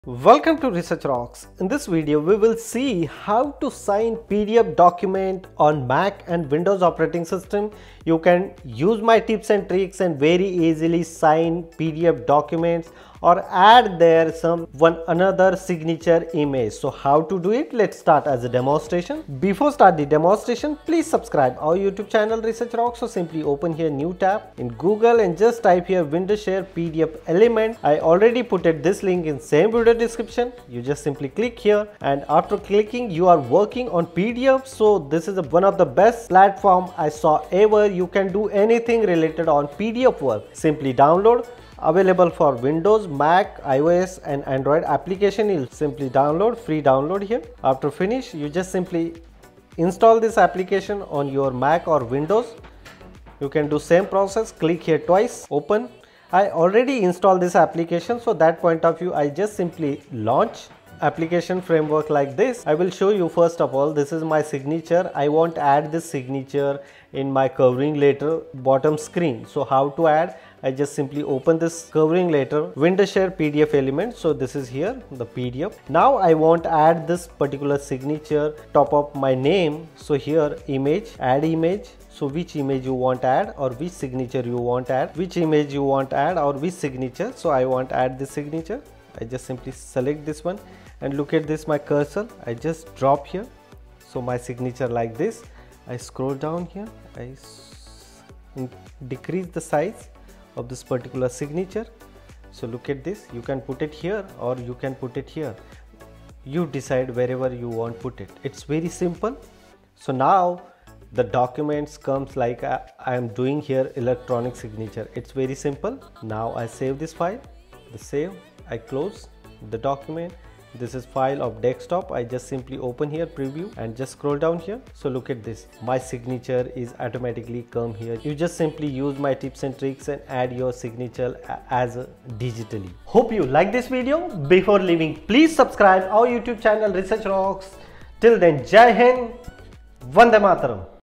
How to Easily Sign PDFs on Mac & Windows | Wondershare PDFelement Tutorial
Learn how to electronically sign PDF documents on both Mac and Windows using Wondershare PDFelement! This step-by-step guide shows you how to add your signature to PDFs quickly and effortlessly. Perfect for professionals, students, or anyone managing digital documents. Save time and streamline your workflow today!